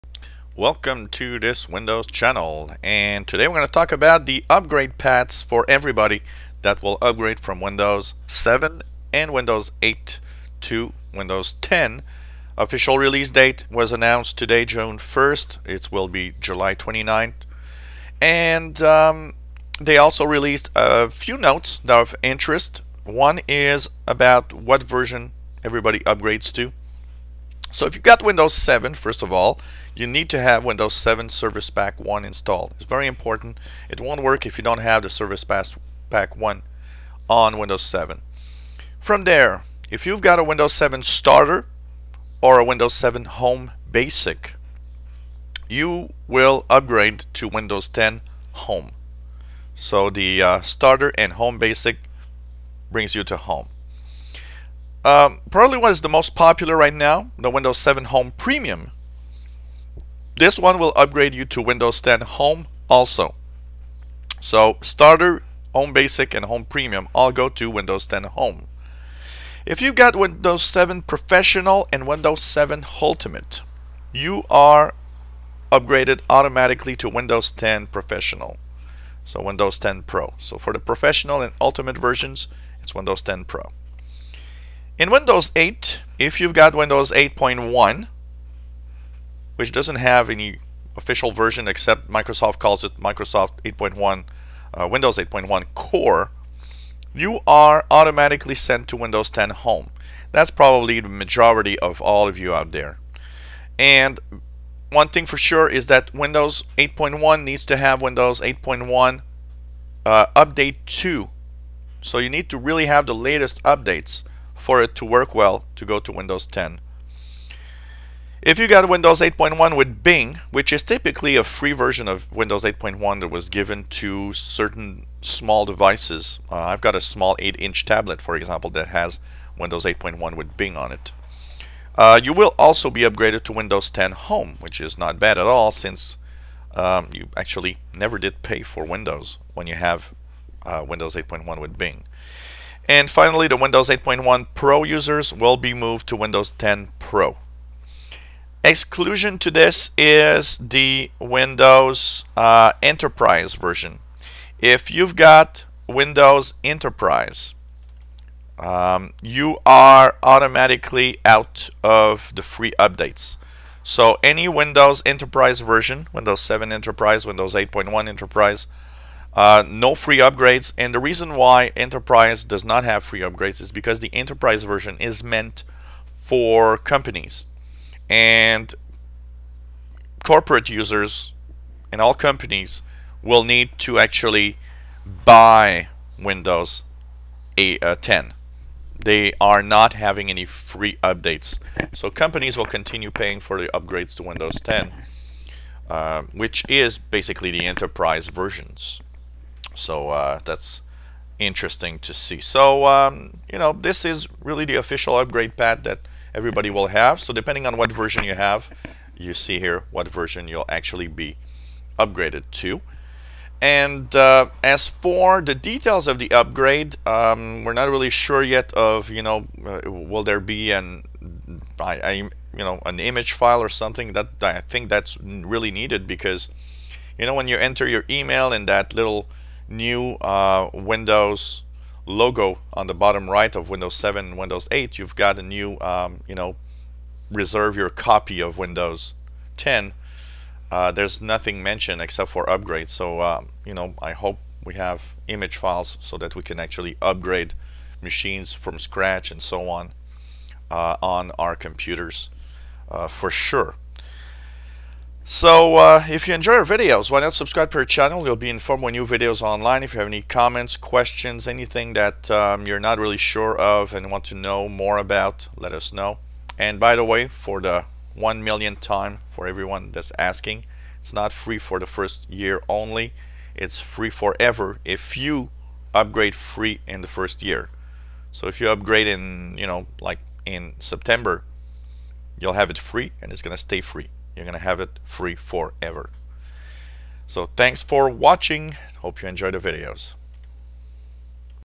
Windows 10 upgrade paths what version will you receive for free
List of the upgrade paths for users of Windows 7 and Windows 8.1 that will upgrade to Windows 10